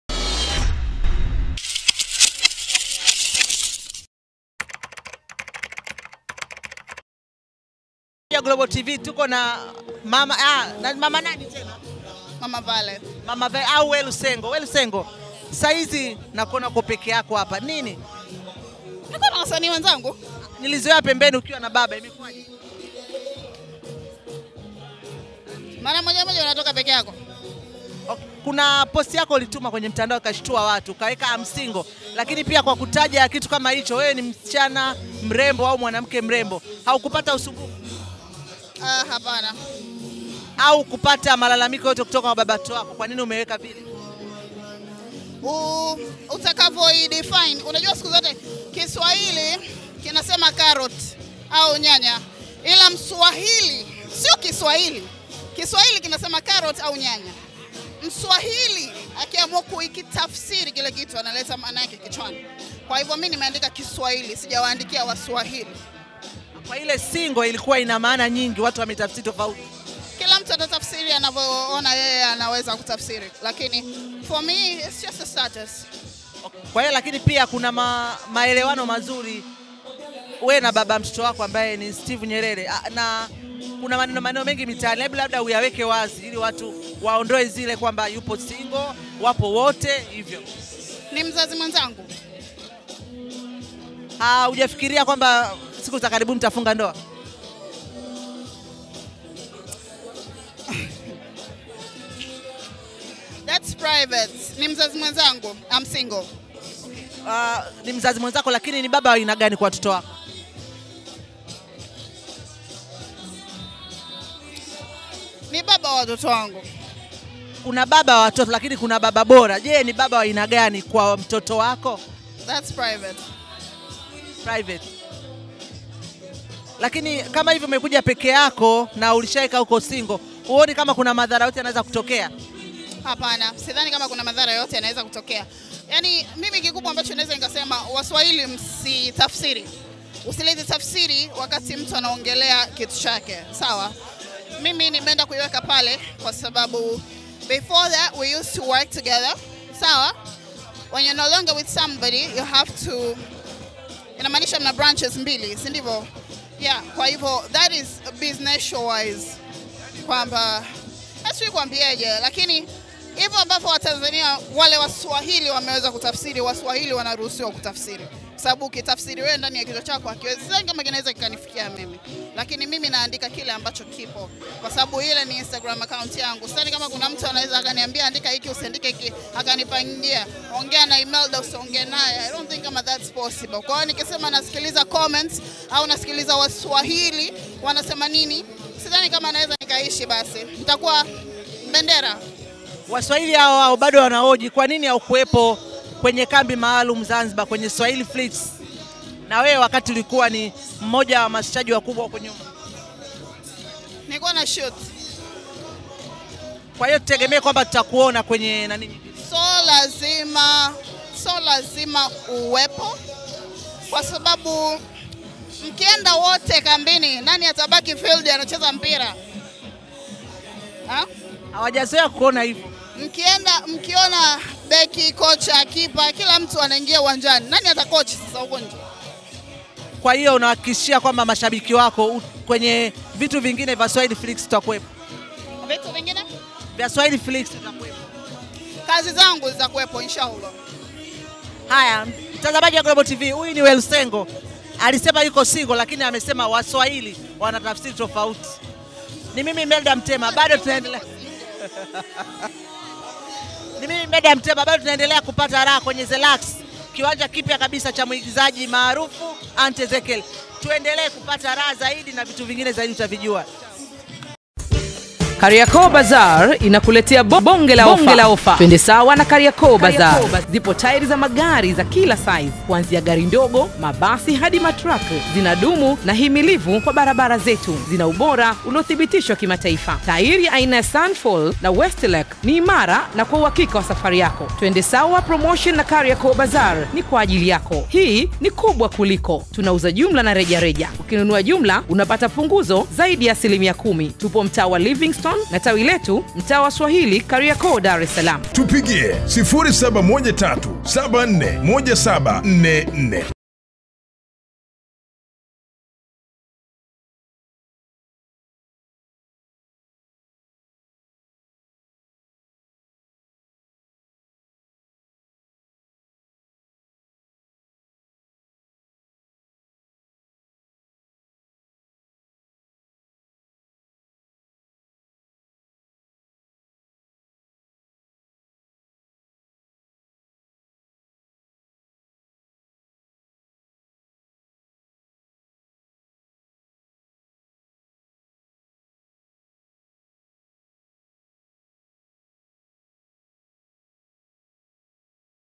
WELLU SENGO Aweka WAZI Kuhusu STEVE NYERERE ''Ndoa sio lazima"
Msaniiwa maigizo Welu Sengo ambaye ni mzazi mwenza na Steve Nyerere  amefunguka na kusema kuwa Steve Nyerere ni mzazi mwenzake na alivyoulizwa kuhusu ndoa akasema " sio lazima"